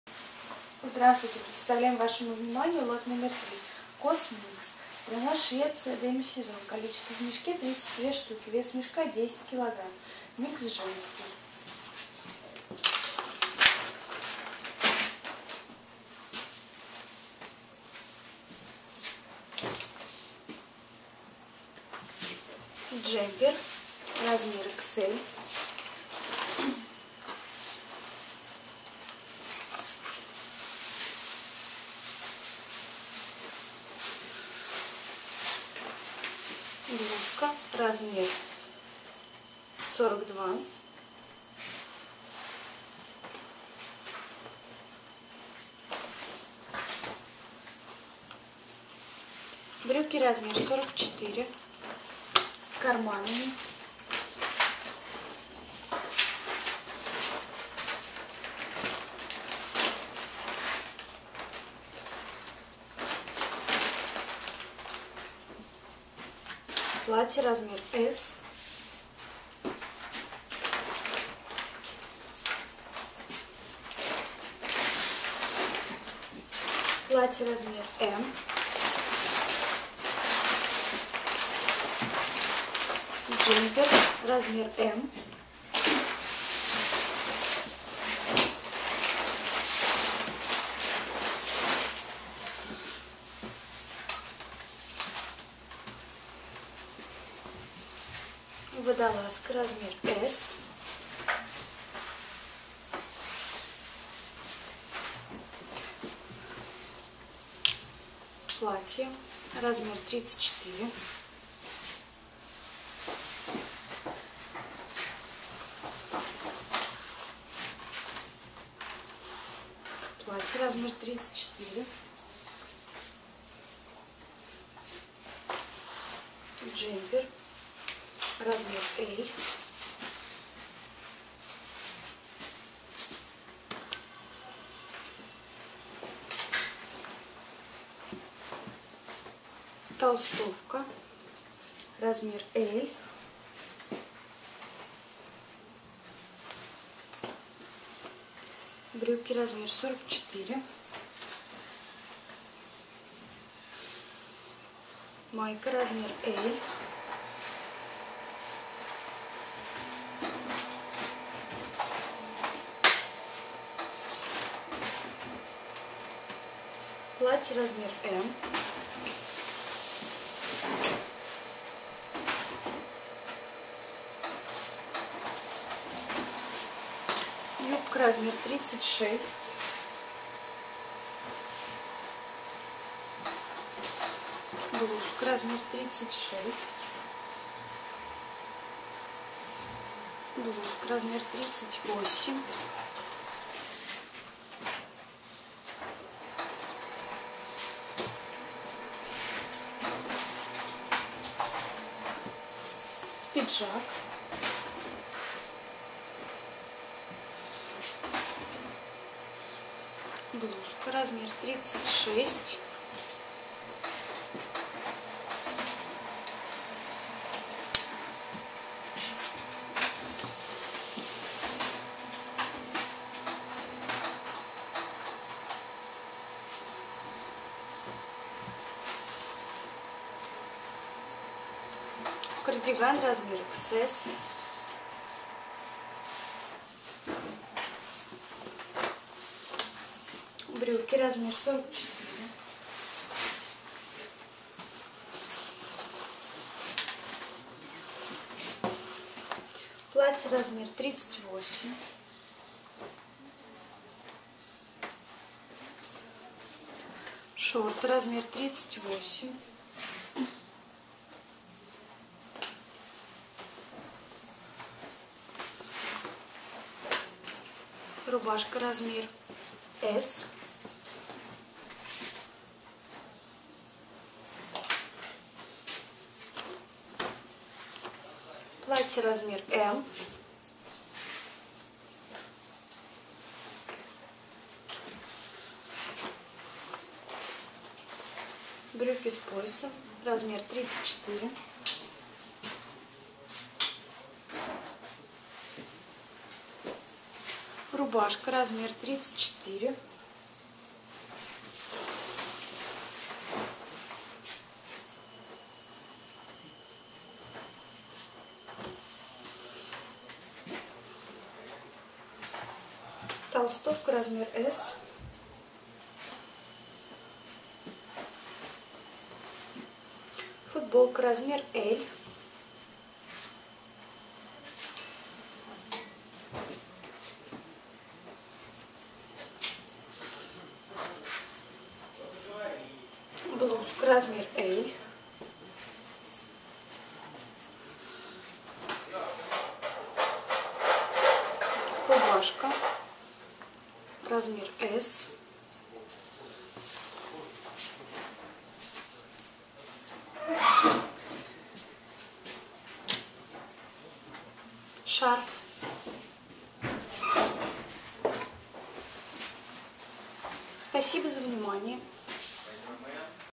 COS mix 3,сток одежда оптом
COS mix (Лот №3)
Женский микс (сток)
Страна: Швеция
Демисезон
Цена за кг.: 43.03 евро
Цена за мешок: 430.3 евро
Цена за ед.: 13.44 евро